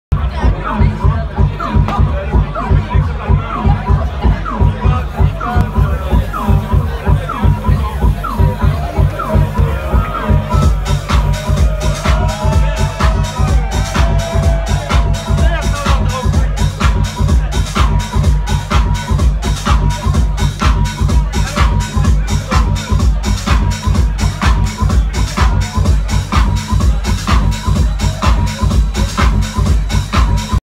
Roomies w/ Fernando Costantini at fuse club (Brussels)
Roomies at fuse club brussels on January 30th with Pierre, Fernando Costantini & John Dimas.